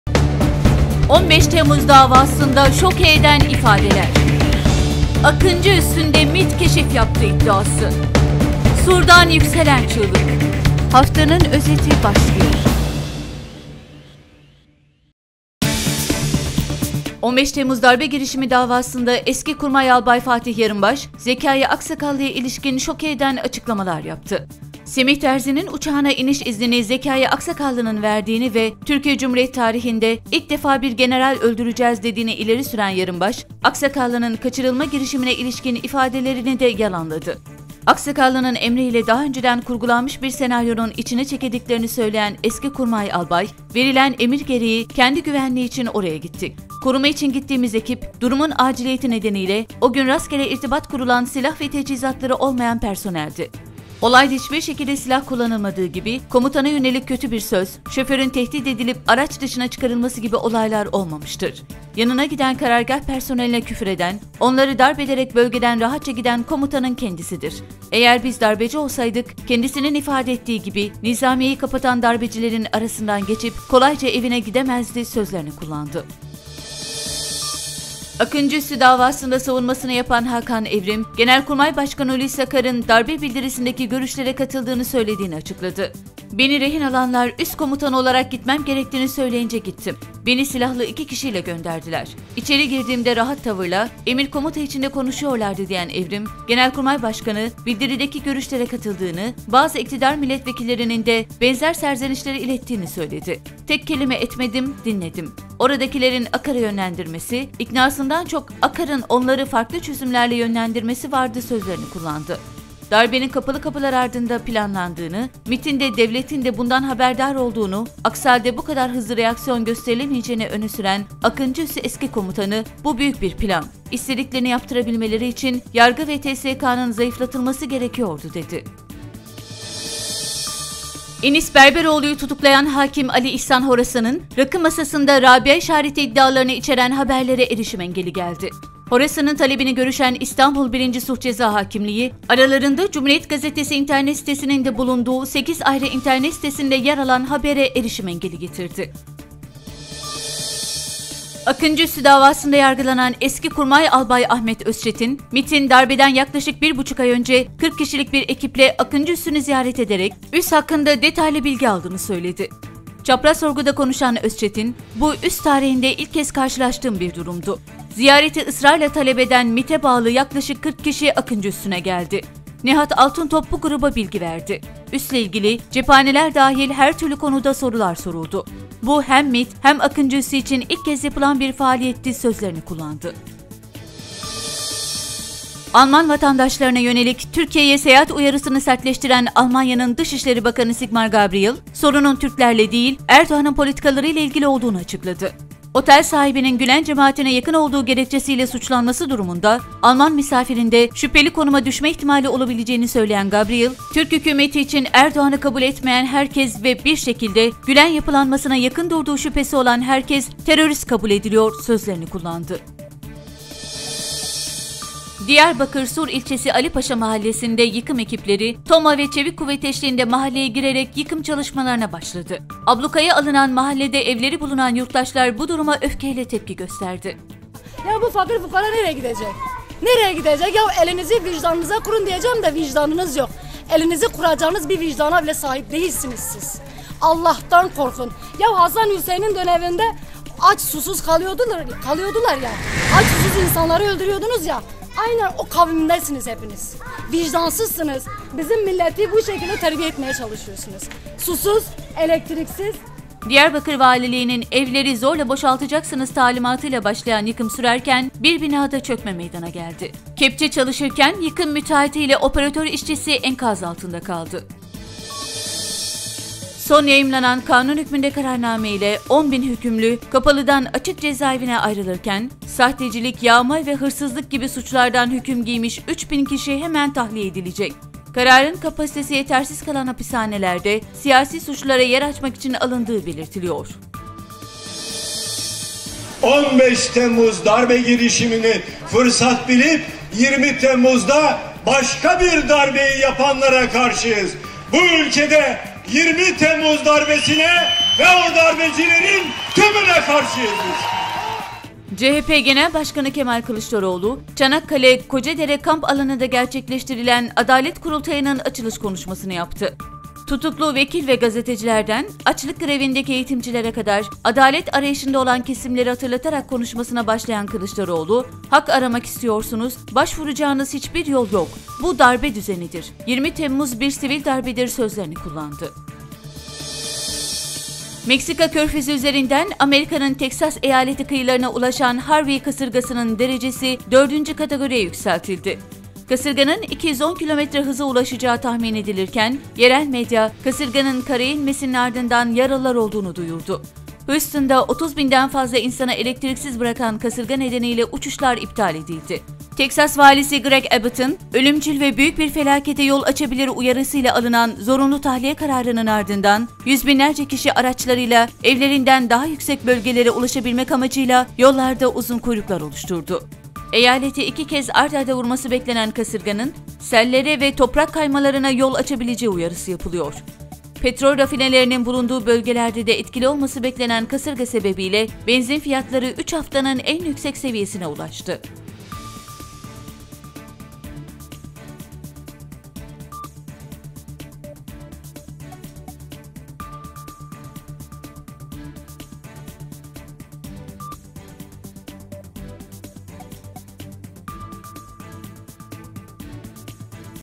Aksakallı'dan şok sözler…‘Akar rehin değildi’…MİT Akıncı’da…Sur’da yıkım…ABD’de kasırga… Hafta Özeti
15 Temmuz darbe girişimi davasında eski Kurmay Albay Fatih Yarımbaş, Zekai Aksakallı’ya ilişkin şoke eden açıklamalar yaptı. ‘Semih Terzi’nin uçağına iniş iznini Zekai Aksakallı’nın verdiğini ve “Türkiye Cumhuriyet tarihinde ilk defa bir general öldüreceğiz” dediğini ileri süren Yarımbaş, Aksakallı'nın kaçırılma girişimine ilişkin ifadelerini de yalanladı. Aksakallı'nın emri ile daha önceden kurgulanmış bir senaryonun içine çekildiklerini söyleyen eski kurmay Albay, ‘Koruma için gittiğimiz ekip, durumun aciliyeti nedeniyle o gün rastgele irtibat kurulan silah ve teçhizatları olmayan personeldi. Olayda hiçbir şekilde silah kullanılmadığı gibi komutana yönelik kötü bir söz, şoförün tehdit edilip araç dışına çıkarılması gibi olaylar olmamıştır. Yanına giden karargah personeline küfür eden, onları darp ederek bölgeden rahatça giden komutanın kendisidir. Eğer biz darbeci olsaydık, kendisinin ifade ettiği gibi nizamiyeyi kapatan darbecilerin arasından geçip kolayca evine gidemezdi.’ sözlerini kullandı.

Akıncı Üssü davasında savunmasını yapan Hakan Evrim, Genelkurmay Başkanı Hulusi Akar’ın darbe bildirisindeki görüşlere katıldığını söylediğini açıkladı. ‘Beni rehin alanlar üs komutanı olarak gitmem gerektiğini söyleyince gittim. Beni silahlı iki kişiyle gönderdiler. İçeri girdiğimde rahat tavırla, emir komuta içinde konuşuyorlardı’ diyen Evrim, Genelkurmay Başkanı, bildirideki görüşlere katıldığını, bazı iktidar milletvekillerinin de benzer serzenişleri ilettiğini söyledi. Tek kelime etmedim, dinledim. Oradakilerin Akar’ı yönlendirmesi, iknasından çok Akar’ın onları farklı çözümlerle yönlendirmesi vardı’ sözlerini kullandı. Darbenin kapalı kapılar ardında planlandığını, MİT'in de devletin de bundan haberdar olduğunu aksi halde bu kadar hızlı reaksiyon gösterilemeyeceğini öne süren Akıncı Üssü eski komutanı "Bu büyük bir plan. istediklerini yaptırabilmeleri için yargı ve TSK'nın zayıflatılması gerekiyordu" dedi.

Enis Berberoğlu'nu tutuklayan hakim Ali İhsan Horasan'ın 'Rakı masasında Rabia işareti' iddialarını içeren haberlere erişim engeli geldi. Horasan’ın talebini görüşen İstanbul 1’inci Sulh Ceza Hakimliği, aralarında Cumhuriyet gazetesi internet sitesinin de bulunduğu 8 ayrı internet sitesinde yer alan habere erişim engeli getirdi.

Akıncı Üssü davasında yargılanan eski Kurmay Albay Ahmet Özçetin, MİT’in, darbeden yaklaşık 1.5 ay önce, 40 kişilik bir ekiple Akıncı Üssü’nü ziyaret ederek, üst hakkında detaylı bilgi aldığını söyledi. Çapraz sorguda konuşan Özçetin, ‘Ziyareti ısrarla talep eden MİT’e bağlı yaklaşık 40 kişi Akıncı Üssü’ne geldi. Üsle ilgili, cephaneler dahil her türlü konuda sorular soruldu. Bu, hem MİT hem Akıncı Üssü için ilk kez yapılan bir faaliyetti” sözlerini kullandı.

Alman vatandaşlarına yönelik Türkiye’ye seyahat uyarısını sertleştiren Almanya’nın Dışişleri Bakanı Sigmar Gabriel, sorunun Türklerle değil Erdoğan’ın politikalarıyla ilgili olduğunuaçıkladı. Otel sahibinin Gülen cemaatine yakın olduğu gerekçesiyle suçlanması durumunda, Alman misafirin de şüpheli konumuna düşme ihtimali olabileceğini söyleyen Gabriel, “Türk hükümeti için Erdoğan’ı kabul etmeyen herkes ve bir şekilde Gülen yapılanmasına yakın durduğu şüphesi olan herkes terörist kabul ediliyor” sözlerini kullandı.

Diyarbakır Sur ilçesi Ali Paşa Mahallesi'nde yıkım ekipleri ve TOMA ve çevik kuvvet eşliğinde mahalleye girerek yıkıma çalışmalarını başladı. Ablukaya alınan mahallede evleri bulunan yurttaşlar bu duruma öfkeyle tepki gösterdi. Diyarbakır valiliğinin ‘evleri zorla boşaltacaksınız’ talimatıyla başlayan yıkım sürerken bir binada çökme meydana geldi. Kepçe çalışırken yıkım müteahhidi ile operatör işçisi enkaz altında kaldı.

CHP Genel Başkanı Kemal Kılıçdaroğlu, Çanakkale Kocadere Kamp Alanı’nda gerçekleştirilen Adalet Kurultayı’nın açılış konuşmasını yaptı. Tutuklu vekil ve gazetecilerden açlık grevindeki eğitimcilere kadar adalet arayışında olan kesimleri hatırlatarak konuşmasına başlayan Kılıçdaroğlu, ‘Hak aramak istiyorsunuz başvuracağınız hiçbir yol yok. Bu darbe düzenidir. 20 Temmuz bir sivil darbedir’ sözlerini kullandı.

Meksika Körfezi üzerinden Amerika'nın Teksas eyaleti kıyılarına ulaşan Harvey Kasırgası'nın derecesi 'dördüncü kategori'ye yükseltildi. Kasırganın 210 kilometre hıza ulaşacağı tahmin edilirken kasırga nedeniyle Houston'da 30 binden fazla insan elektriksiz kaldı, uçuşlar iptal edildi.

Alınan zorunlu tahliye kararının ardından yüz binlerce kişi araçlarıyla evlerinden daha yüksek bölgelere ulaşabilmek amacıyla yollarda uzun kuyruklar oluşturdu. Eyaleti iki kez art arda vurması beklenen kasırganın sellere ve toprak kaymalarına yol açabileceği uyarısı yapılıyor. Petrol rafinerilerinin bulunduğu bölgelerde de etkili olması beklenen kasırga sebebiyle benzin fiyatları üç haftanın en yüksek seviyesine ulaştı.